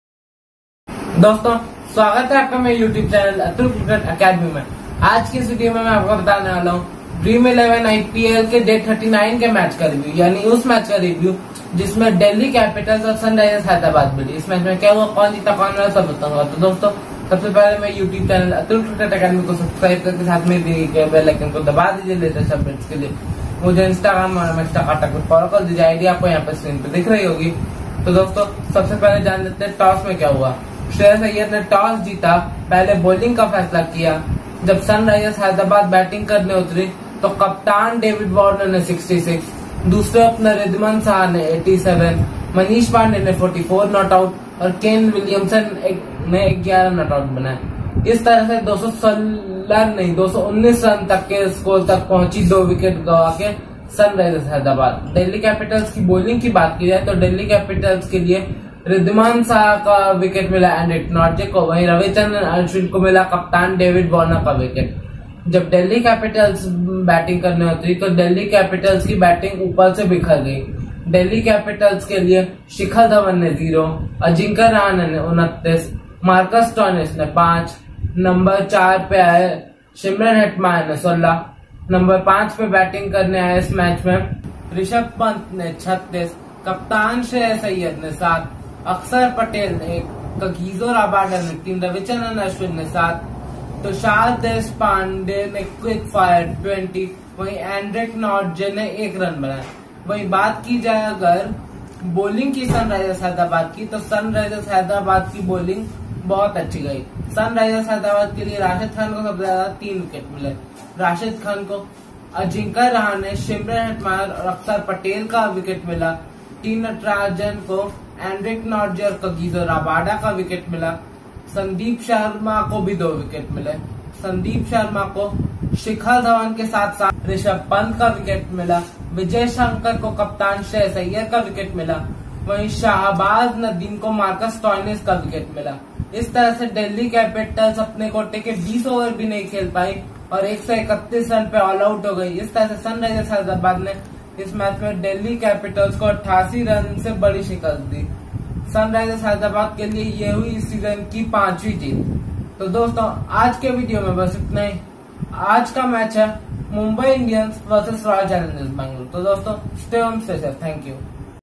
IPL 2020 Day 39 review Review of match between Delhi capitals and Sunrisers Hyderabad .
MX takatak ID
Atul Cricket Academy
Instagram ID
Atul Cricket Academy

Cameraman Akshika Singh
You Tuber Atulsingh
Atul Cricket Academy